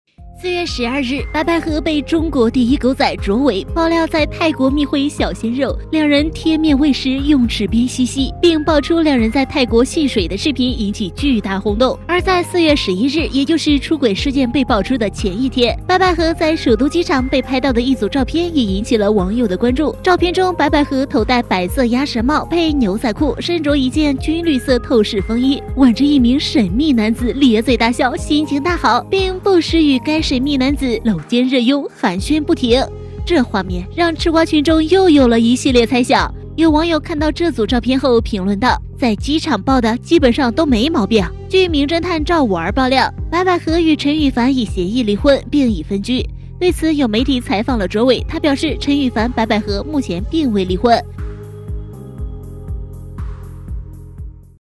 【3年前】白百何出事前一天摟神秘男 摟肩熱擁寒暄不停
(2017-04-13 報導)

4月11日，也就是出軌事件被曝出的前一天，白百何在首都機場被拍到的一組照片引起了網友的關注。照片中白百何頭戴白色鴨舌帽配牛仔褲，身著一件軍綠色透視風衣，挽著一名神秘男子，咧嘴大笑，心情大好，并不時與該神秘男子摟肩熱擁，寒暄不停。